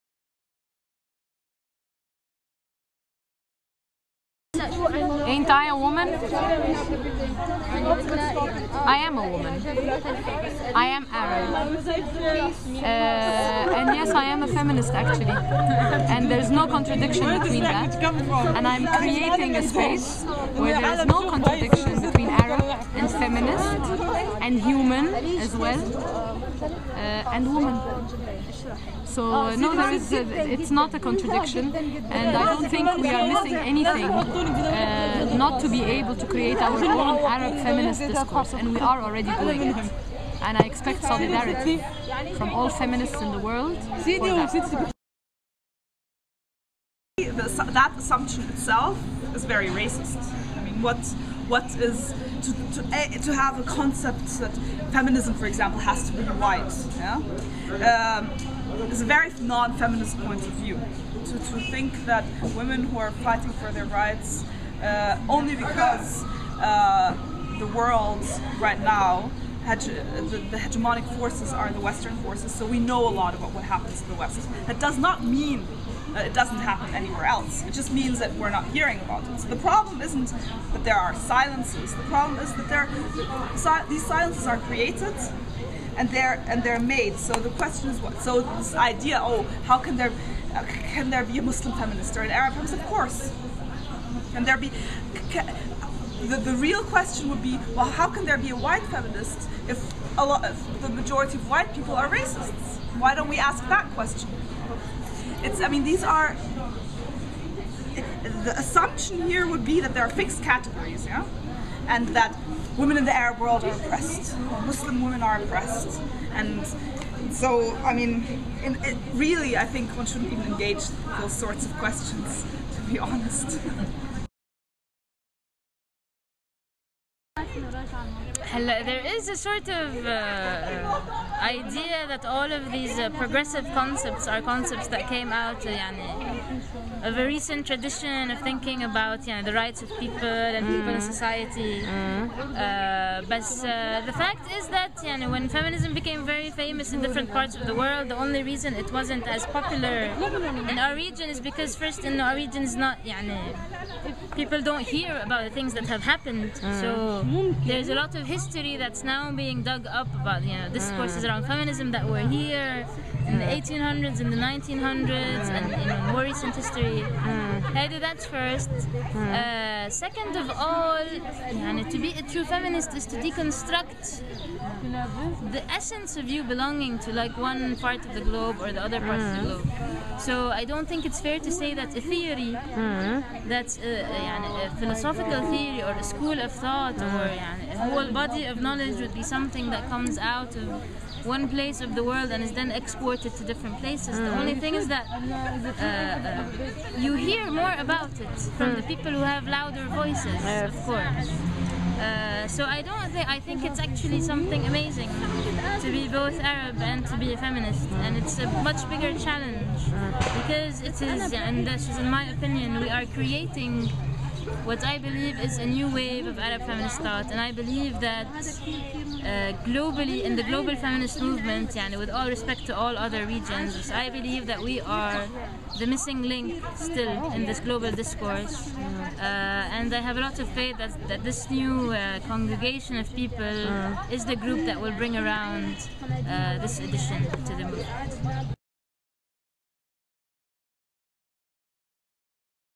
Is being an Arab and a feminsit a contradiction?
From April 29, 2010 till May 2, 2010, 20 participants from across seven Arab countries gathered in Cairo for a four-day meeting to kick off the first Young Arab Feminist Network (YAFN).

Our feminist webspace Sawt Al Niswa was there to ask them about their thoughts on being Arab and feminists.